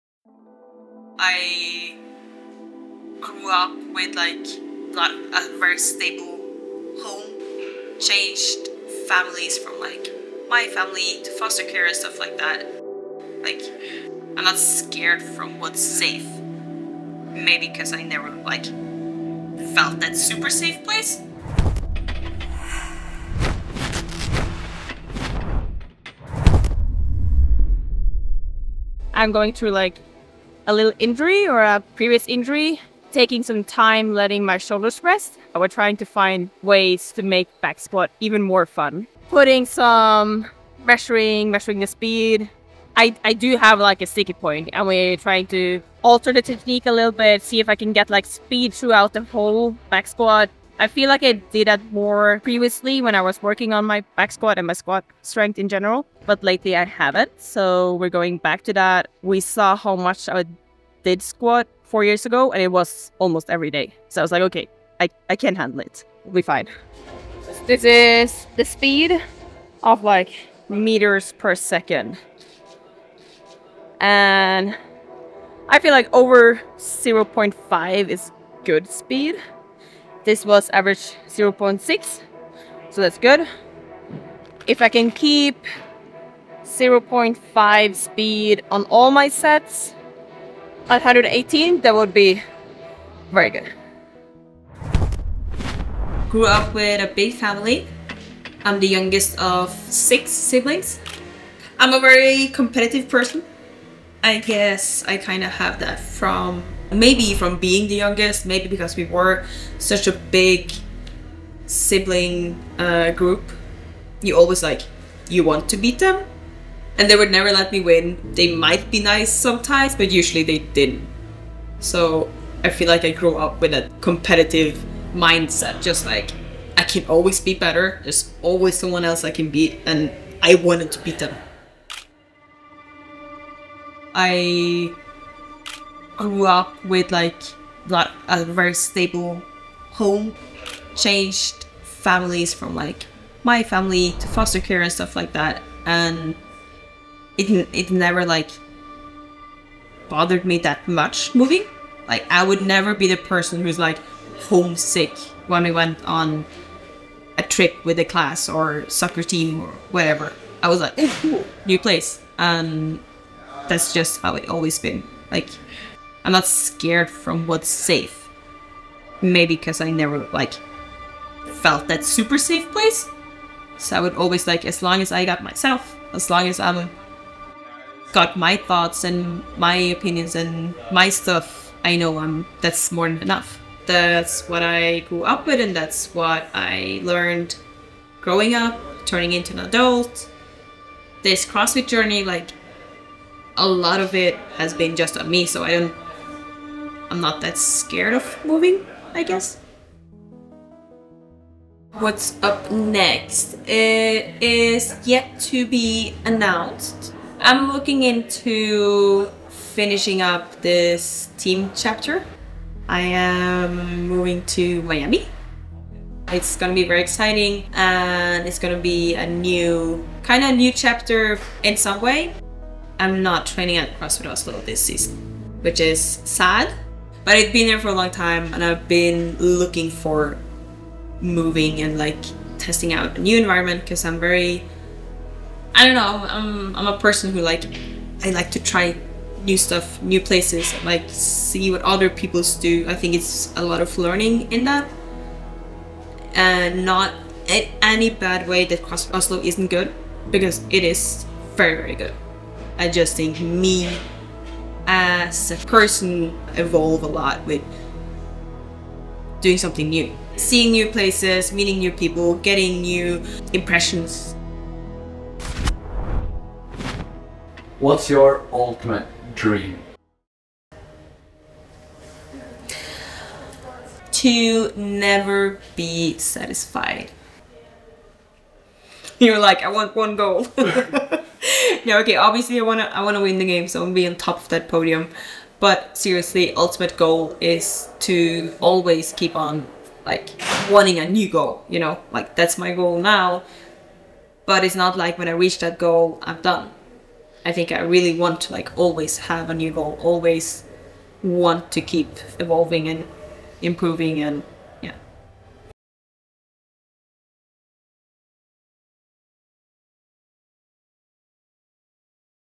3x Podium CrossFit Games Athlete: I wanna win The Games!
In this raw and honest video, Lena Richter shares some of her personal story, from a less stable home environment to navigating the world of competitive CrossFit.

Growing up as the youngest of six siblings, Lena Richter developed a competitive mindset that has driven her to constantly strive for improvement. Her goal; to win the CrossFit Games.
We get a peak in to her training where she uses technology to measure speed to make necessary adjustments to enhance performance, even while overcoming a recent injury.

Exciting news awaits as Lena Richter reveals her upcoming move to Miami, a decision driven by a thirst for new experiences and personal growth.